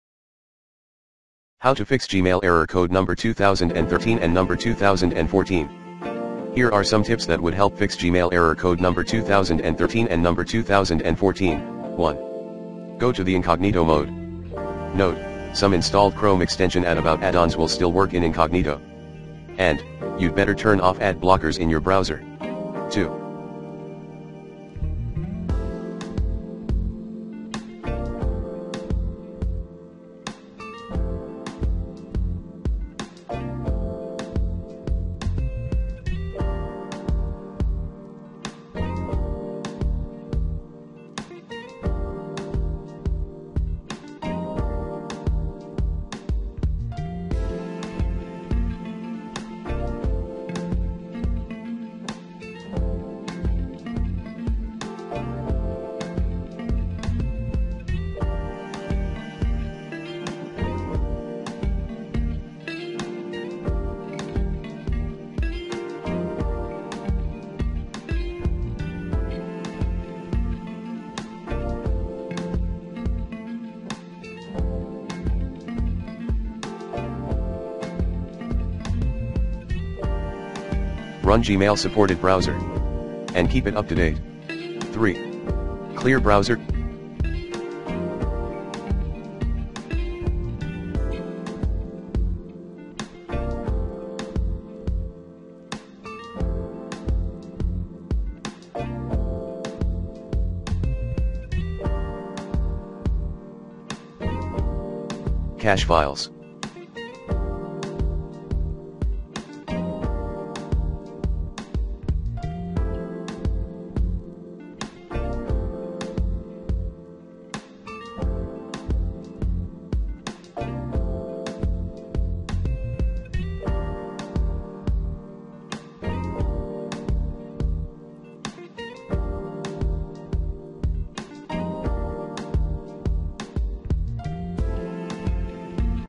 Fix Gmail Error Code 2013/2014 (2024 updated)
Here's how to Fix Gmail Error Code 2013/2014.

Gmail error codes 2013 and 2014 typically occur when there's an issue with accessing or syncing Gmail data in an email client or app. These errors can be frustrating, but there are several troubleshooting steps you can take to resolve them. Here are detailed tips for fixing Gmail error codes 2013 and 2014:

1. **Check Gmail Service Status**:
   - Before troubleshooting, make sure Gmail's service is not experiencing any outages or disruptions. You can check the current status of Gmail by visiting the G Suite Status Dashboard or other reliable sources.

2. **Update Your Email Client/App**:
   - Ensure that your email client or app is updated to the latest version. Sometimes, outdated software can cause compatibility issues with Gmail.

3. **Restart Your Device**:
   - Try restarting your computer, smartphone, or tablet. This simple step can often resolve temporary glitches or connectivity issues.

4. **Verify Internet Connection**:
   - Ensure that your device is connected to a stable and reliable internet connection. Poor connectivity or intermittent network issues can cause errors when trying to access Gmail.

5. **Disable VPN or Proxy**:
   - If you're using a VPN (Virtual Private Network) or proxy server, try disabling it temporarily. Sometimes, these network configurations can interfere with Gmail's connectivity.

6. **Clear Cache and Cookies**:
   - Clear the cache and cookies in your web browser or email app. Cached data can sometimes become corrupted and cause errors. After clearing the cache, restart your browser or app and try accessing Gmail again.

7. **Check Account Settings**:
   - Verify that your Gmail account settings are configured correctly in your email client or app. Ensure that the incoming and outgoing server settings, including server addresses, ports, and security protocols (such as SSL/TLS), are accurate.

8. **Disable Antivirus or Firewall**:
   - Temporarily disable any antivirus or firewall software on your device. Sometimes, security software can mistakenly block Gmail's access, causing error codes 2013 and 2014.

9. **Use Web Interface**:
- If you're encountering errors in an email client or app, try accessing Gmail through the web interface This can help determine if the issue is specific to the client/app or if it's a broader problem with your Gmail account.

10. **Check IMAP/POP Settings**:
    - If you're using IMAP or POP to access Gmail, double-check the configuration settings in your email client. Ensure that IMAP/POP access is enabled in your Gmail account settings and that the correct server settings are used.

11. **Contact Support**:
    - If you've tried the above steps and are still experiencing Gmail error codes 2013/2014, consider contacting Gmail support or the support team for your email client/app. They may be able to provide further assistance or diagnose any underlying issues.

By following these detailed tips, you should be able to troubleshoot and resolve Gmail error codes 2013 and 2014 effectively. If the problem persists, reaching out to technical support for additional assistance may be necessary.